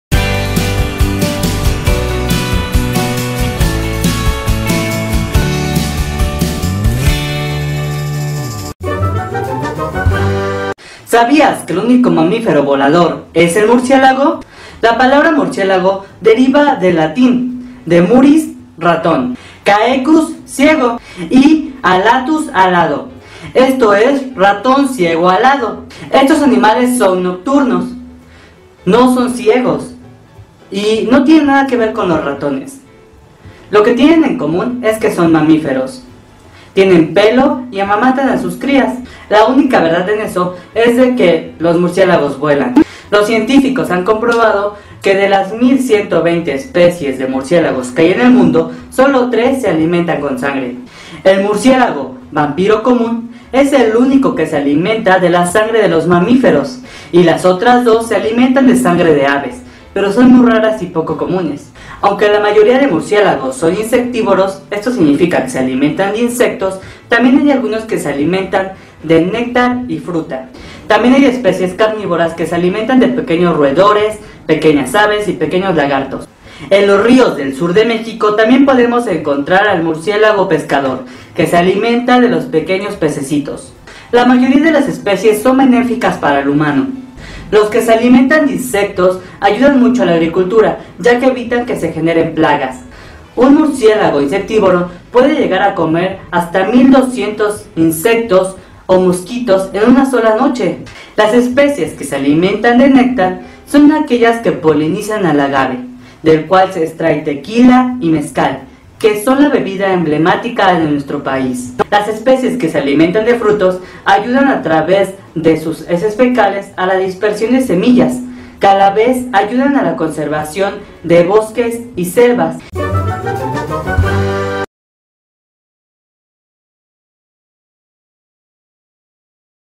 MURCIÉLAGOS|LOS PELUDITOS QUE VUELAN| EN CORTO| PEPE CONCIENCIA
Los murciélagos han tenido una mala  casi todo el tiempo y en estos día por el coronavirus, pero ellos no han sido los causantes, así que mejor debemos conocerlos para quererlos :)